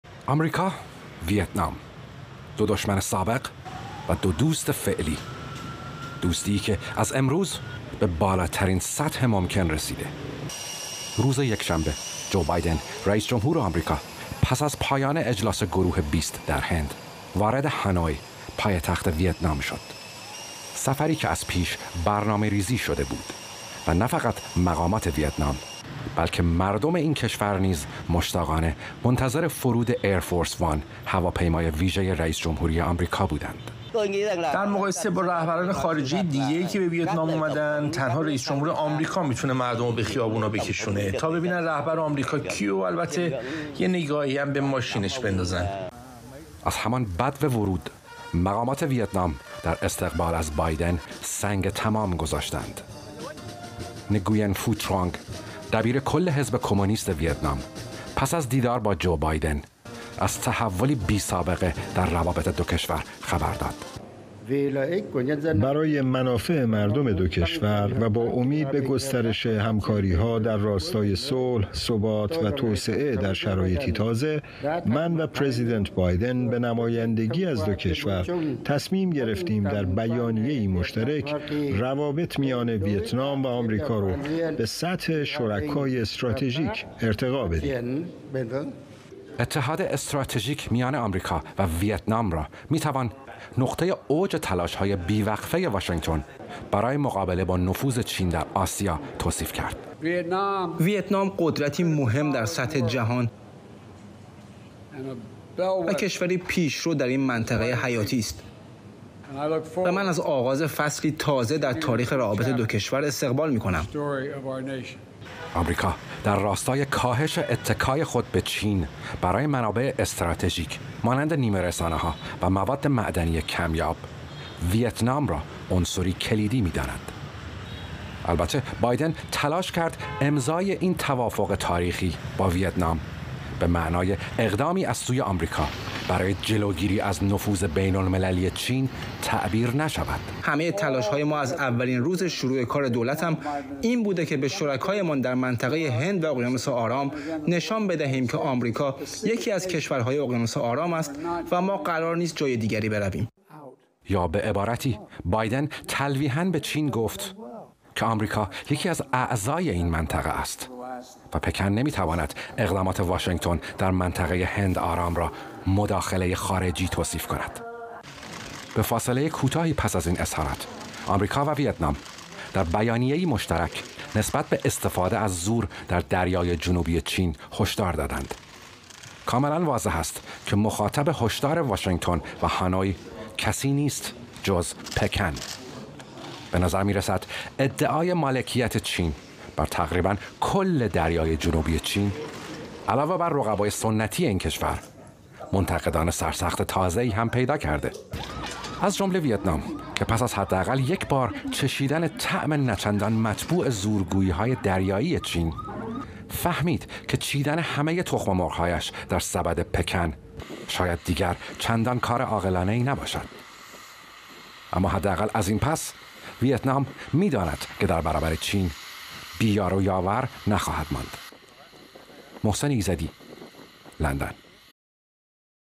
اتحاد آمریکا و ویتنام
اتحاد آمریکا و ویتنام؛ پس از ۵۰ سال آمریکا و ویتنام، دو دشمن سابق، متحد یکدیگر شدند

گزارش محسن ایزدی، ایران اینترنشنال